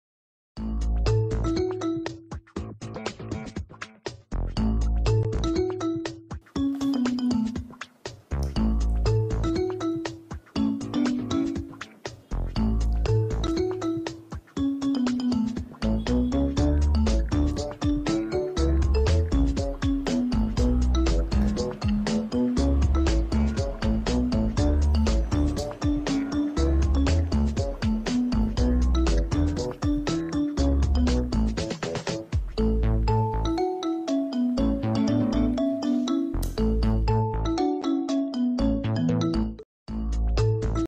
Hello,my name is tsering.i am from nepal.deaf world saw all my youtube.thank you
Tsering lama